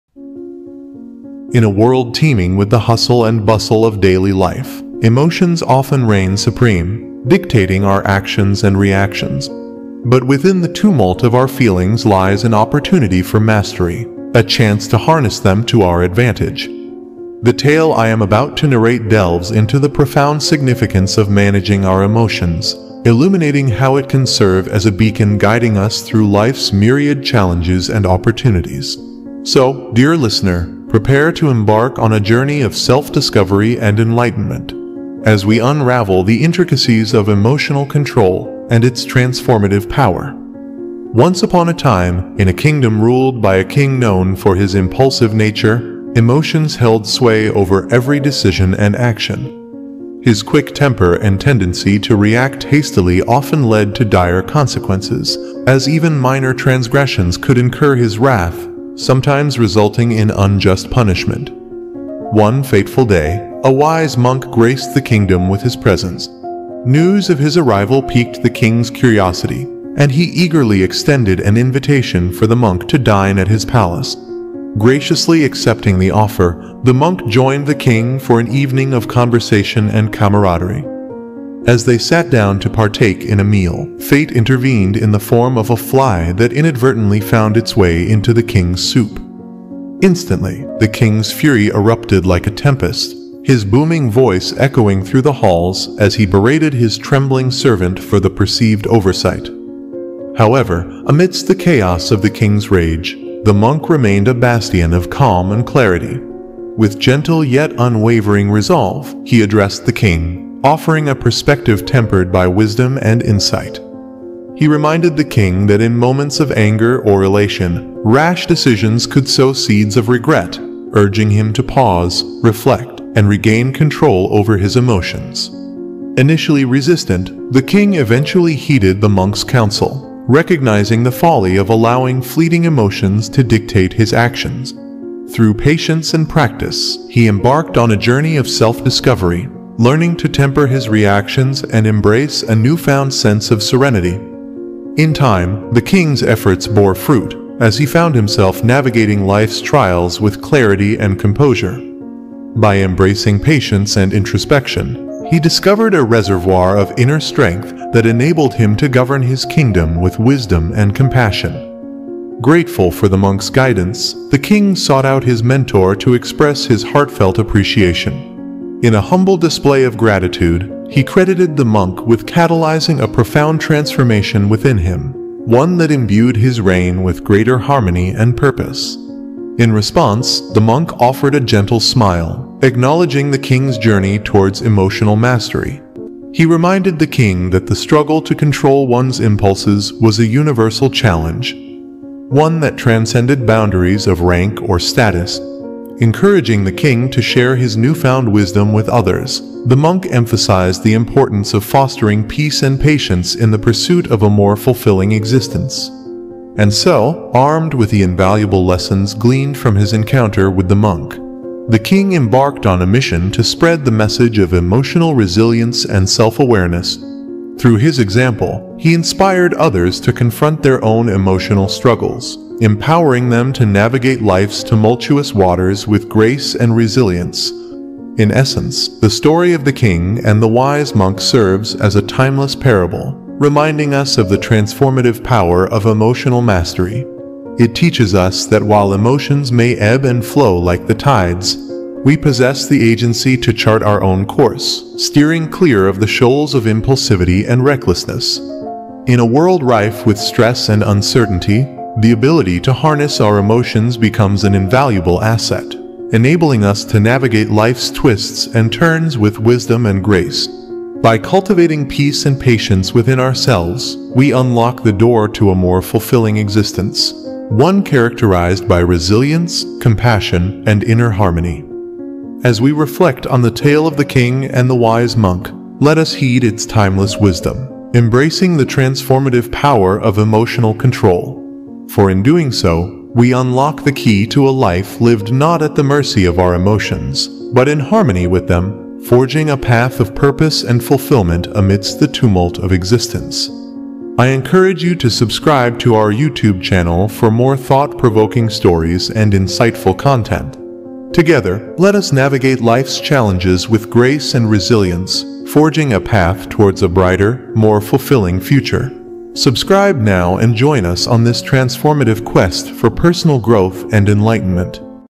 The Power of NOT Reacting | How To Control Your Emotions | Motivational Story | Zen Story
The power of not reacting means waiting for some time before reacting to your emotions during a conversation or situation. This Gautam buddha motivational story will teach you that It's important to learn some self control techniques so that impulsive behavior doesn't get out of our hand! This is where a little bit of psychology comes in handy ! If you're able to stay calm and collected even when someone else triggers you, then you'll be able to better communicate and resolve conflicts more quickly. And that's just one way this skill can help you out!  This video will teach you how to stop overreacting and stay calm when triggered emotionally, so you can start responding rationally instead.